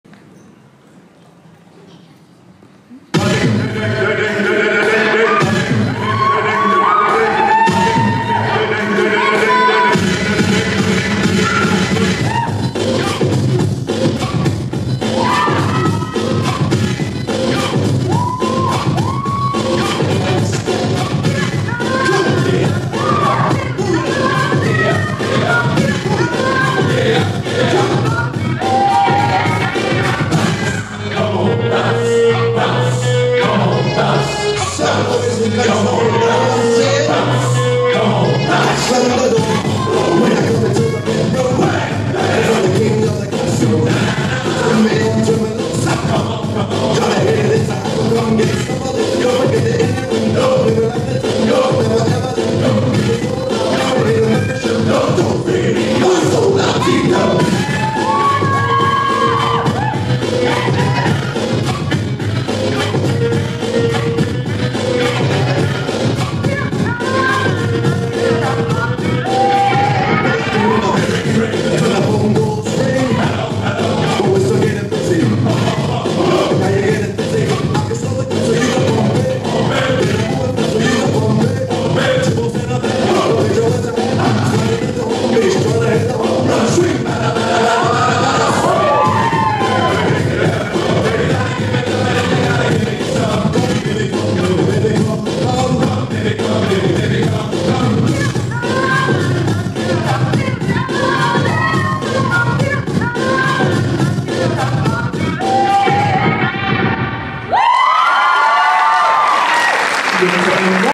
23-24 Qualifier BE - Jinte & Mona (Dance Control)
*Jinte & Mona from Dance Control in Kortrijk, BE
*First place Hip Hop Juniors Duo/Trio at the qualifier in Menen, BE . On the 4th of May.
*Choreography by Ryanne Vanhulst